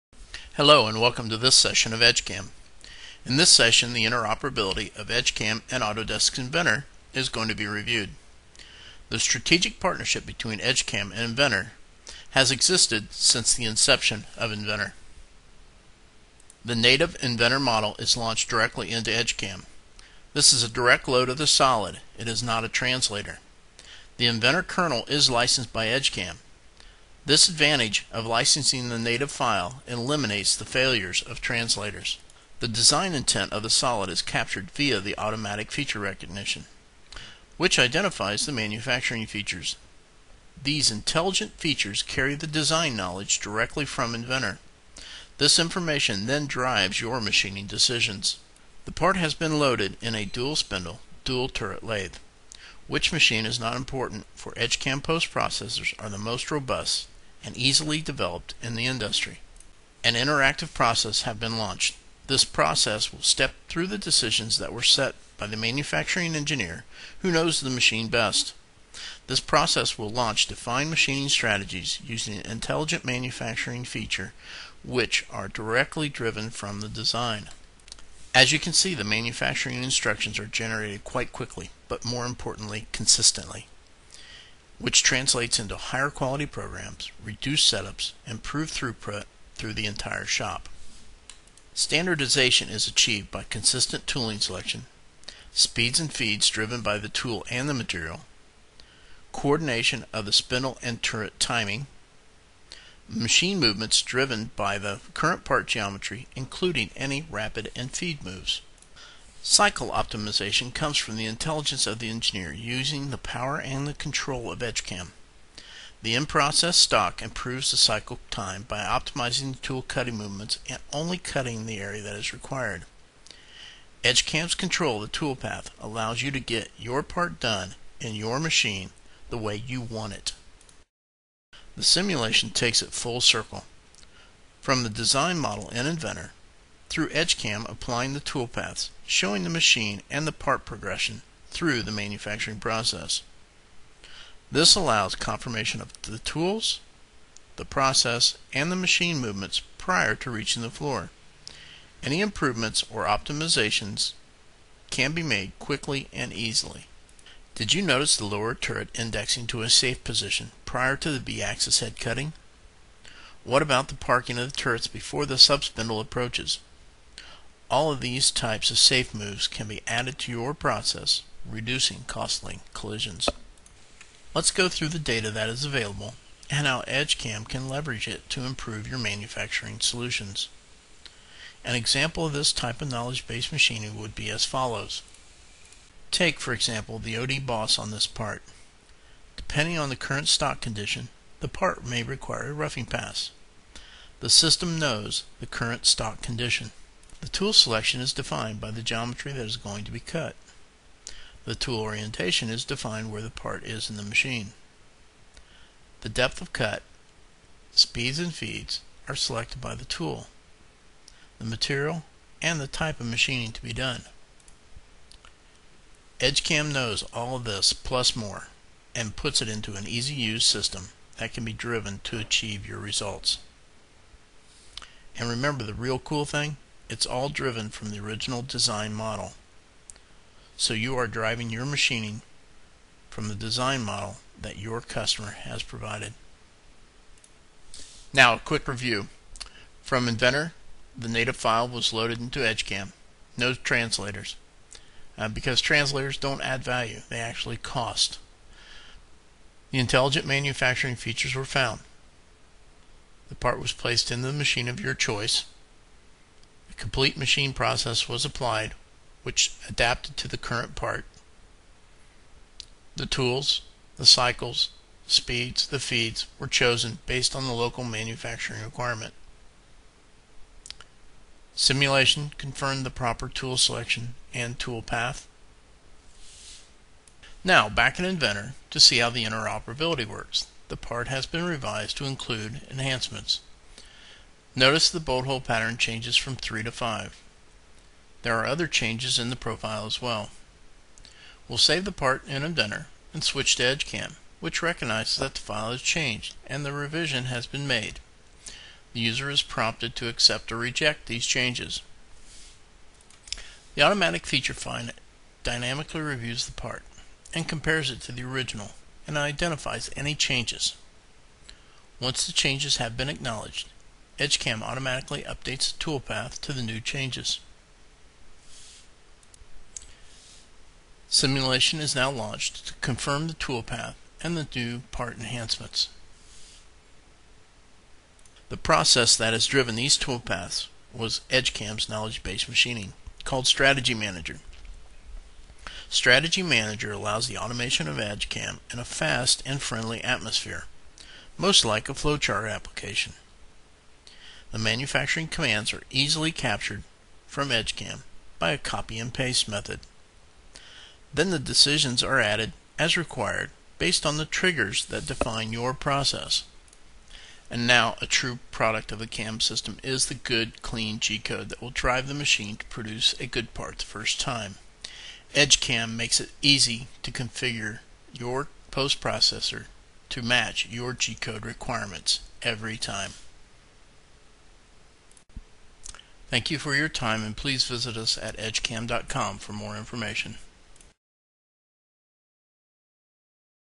Edgecam: Edgecam sluit naadloos aan op Autodesk Inventor
Deze video toont de integratie tussen Edgecam en Autodesk Inventor. In deze video wordt de connectiviteit getoond op basis van een draai/frees onderdeel. Met Edgecam sluit u naadloos aan op uw Autodesk Inventor 3D ontwerpsoftware, dit betekend dat u zonder problemen uw productie snel en betrouwbaar kunt aansturen. Edgecam wordt in de Benelux geleverd door Widenhorn BV gevestigd te Rhoon. Voor meer informatie